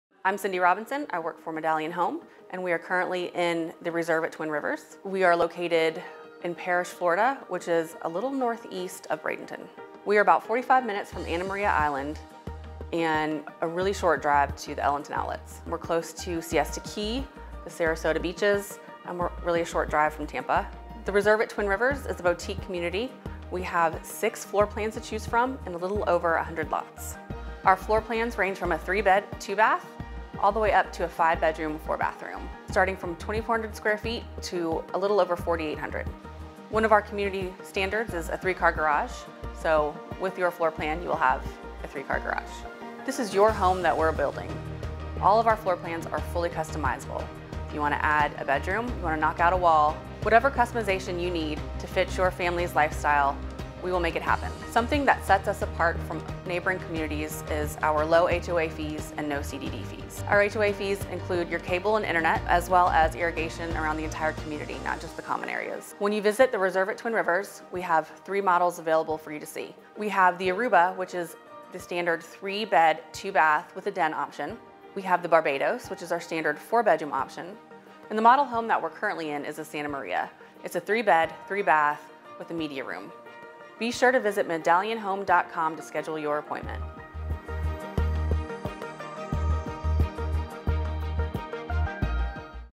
Start Your New Home Journey at The Reserve at Twin Rivers
Nestled in tranquil and peaceful Parrish, a quaint town, yet close to Manatee-Sarasota and St. Petersburg-Tampa metro markets, is The Reserve at Twin Rivers. As a gated community with large lots to choose from, it is the place to call home. All homes come with gourmet kitchen, tile roof, driveway/lead walk and entry pavers and three car garages.